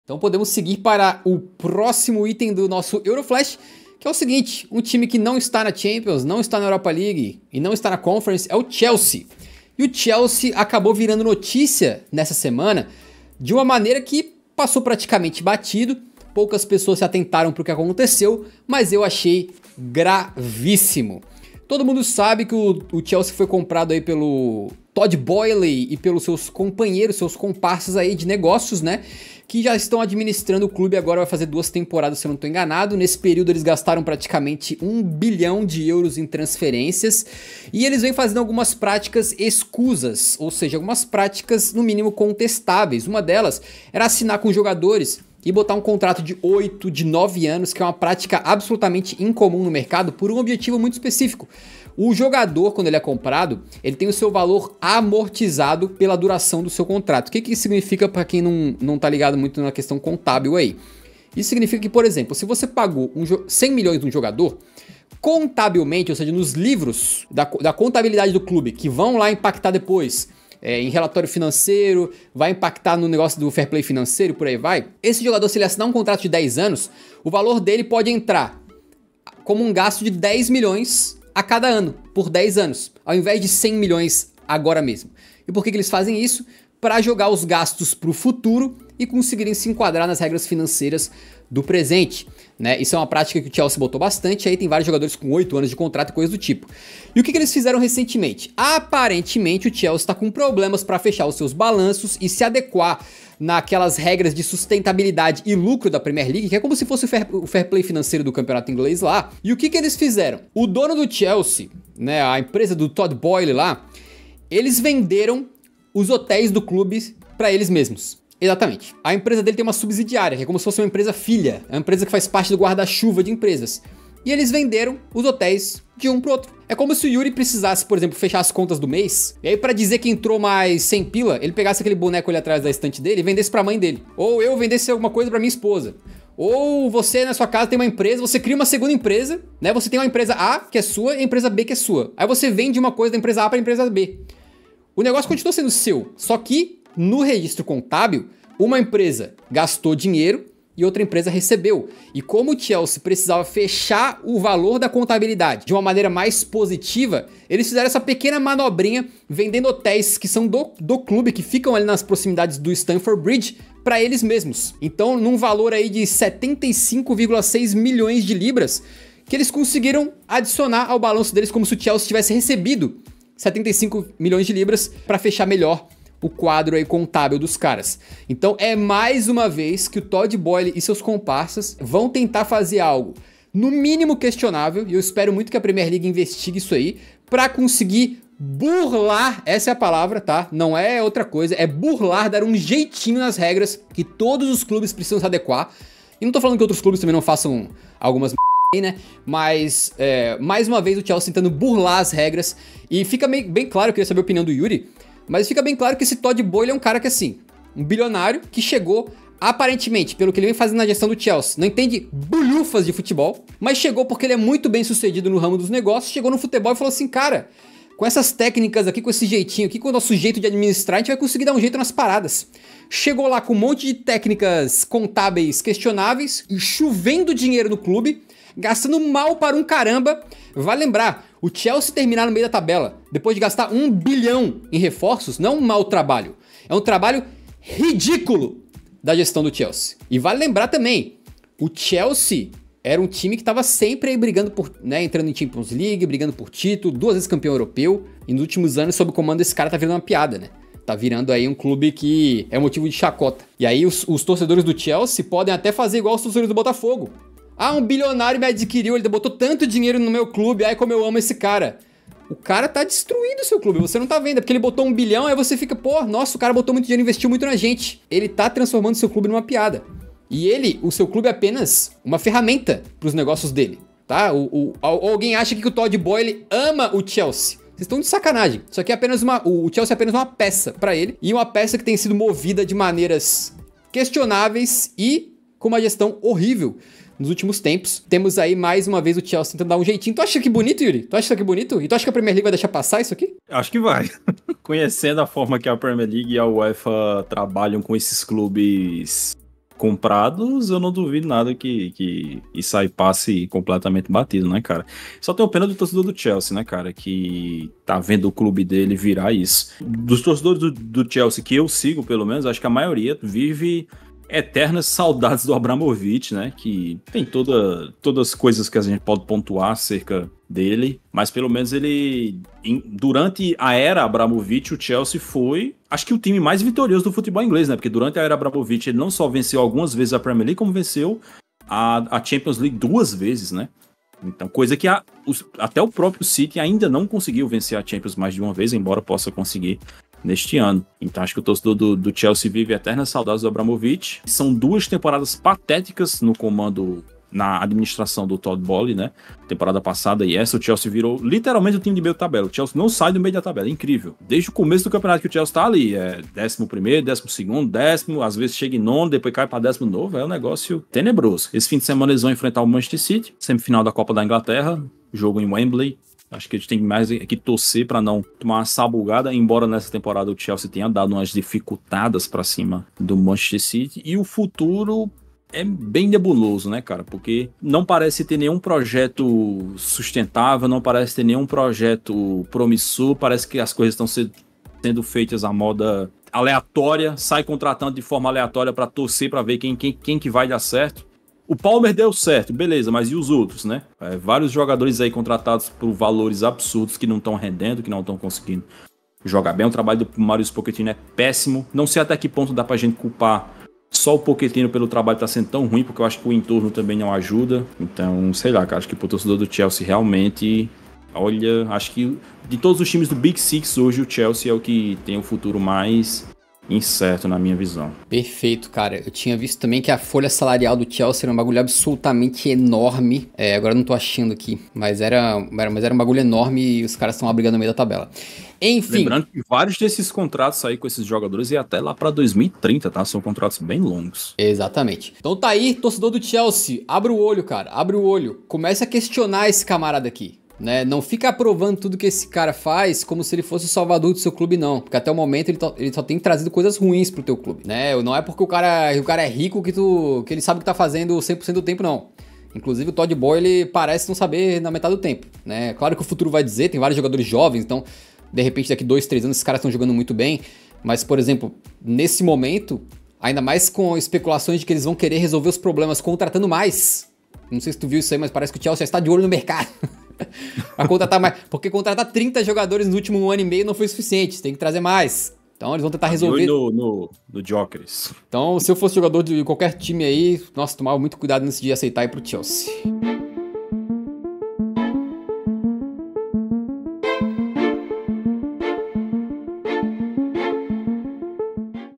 Dono do CHELSEA tenta BURLAR o FAIR PLAY FINANCEIRO (de novo)
Os proprietários do Chelsea aparentemente venderam uns hotéis pra eles mesmo pra resolver o balanço financeiro. Quero ver se vem alguma punição, hein?

🎶 Trilha sonora por Streambeats.com